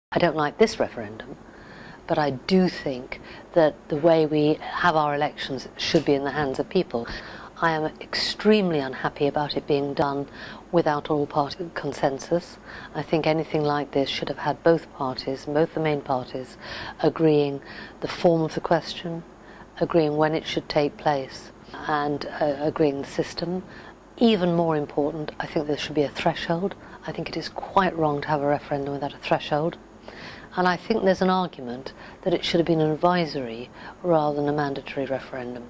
Baroness Hayter - It is right to have a referendum
Dianne Hayter defends the first past the post system in advance of the election on AV.